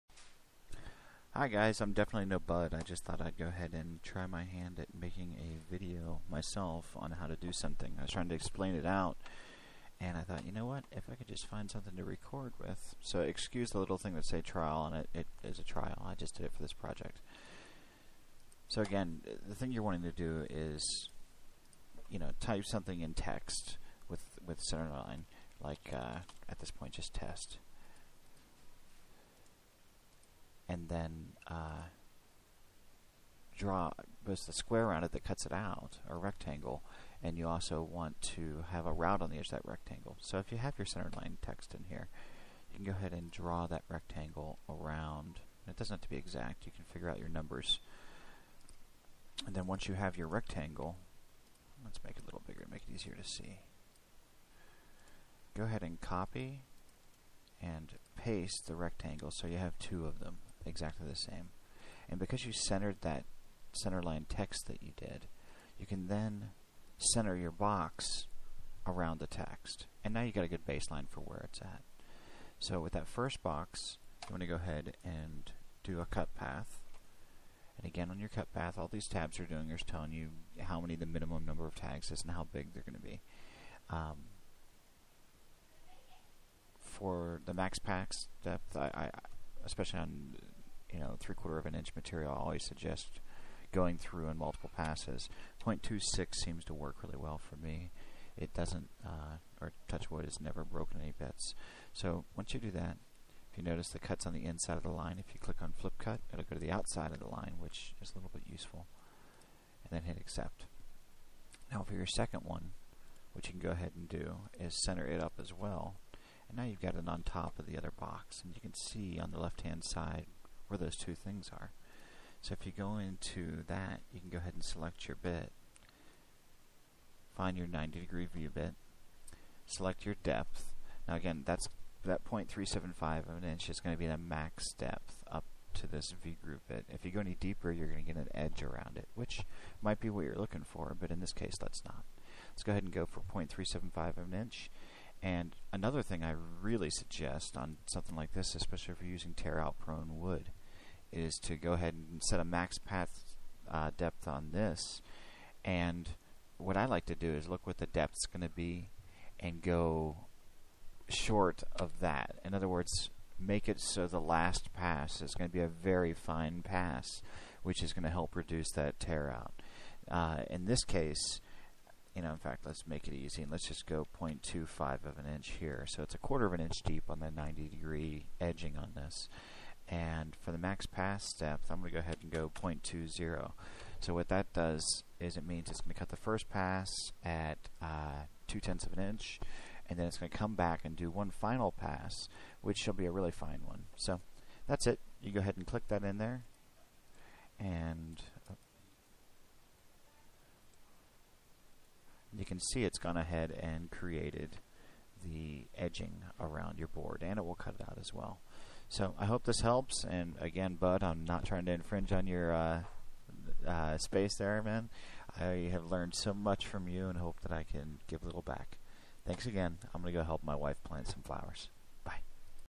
90 degree around square.avi
How to create a cutout and 90 degree chamfer at the same time.